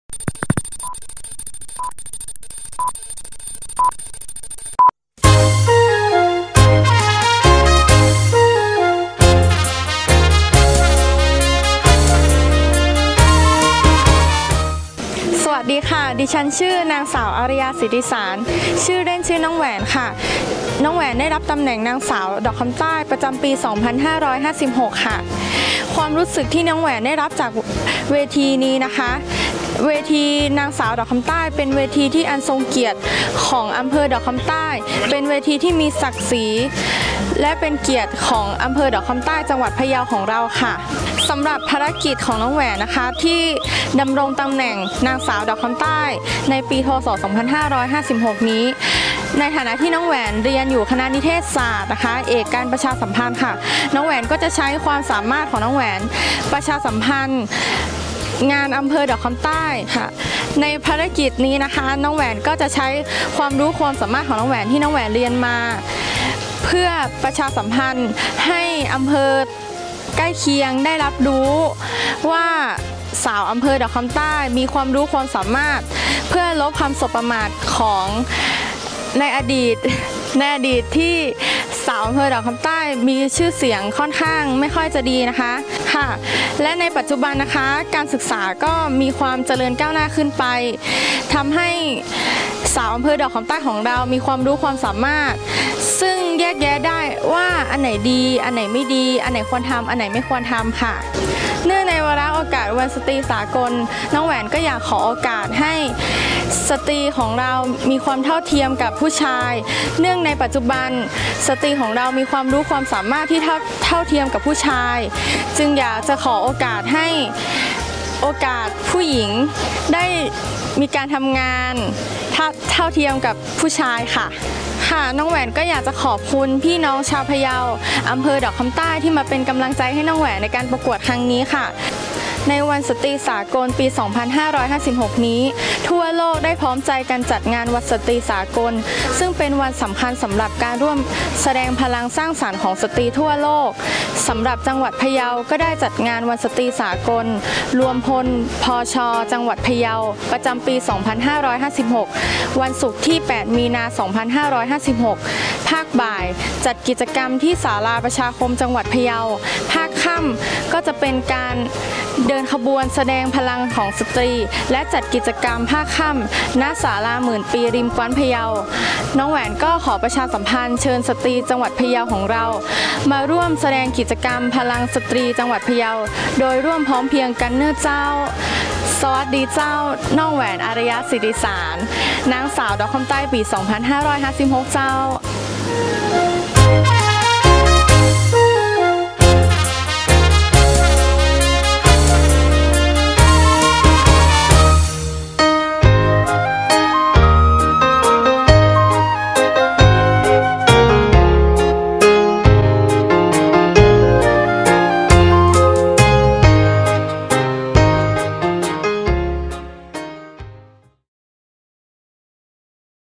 นางสาวดอกคำใต้(อารยา ศีติสาร)
นางสาวดอกคำใต้ ปี ๒๕๕๖ เชิญชวนร่วมงานวันสตรีสากล จ.พะเยา...เมื่อวันพฤหัสบดีที่ ๒๑ กพ.๒๕๕๖ ณ ร้าน Amazon ปั้ม ปตท.บ้านร่องห้า ทีมงานสารสนเทศฯ สนง.พัฒนาชุมชนจังหวัดพะเยา ได้พบปะ และพูดคุยกับ น้องแหวน..อารยา ศีติสาร นางสาวดอกคำใต้ ประจำปี ๒๕๕๖ ในงานวันดอกคำใต้งามและของดีอำเภอดอกคำใต้..น้องแหวนให้สัมภาษณ์ในการปฏิบัติภารกิจของนางสาวดอกคำใต้  ให้ความคิดเห็นต่อบทบาทสตรี เนื่องในโอกาสวันสตรีสากล  และเชิญชวนสตรีจังหวัดพะเยาทุกอำเภอเข้าร่วมกิจกรรมแสดงพลังในวัน "สตรีสากล  รวมพลคน พช." จังหวัดพะเยา ในวันศุกร์ที่ ๘ มีค.๒๕๕๖ ภาคบ่าย..ณ ศาลาประชาคมจังหวัดพะเยา และภาคค่ำ...ณ ศาลาหมื่นปี  ริมกว๊านพะเยา..ขอขอบคุณน้องแหวน และทีมงานไว้ ณ โอกาสนี้ด้วย..น้องน่ารักและยังใจดีอีกด้วย..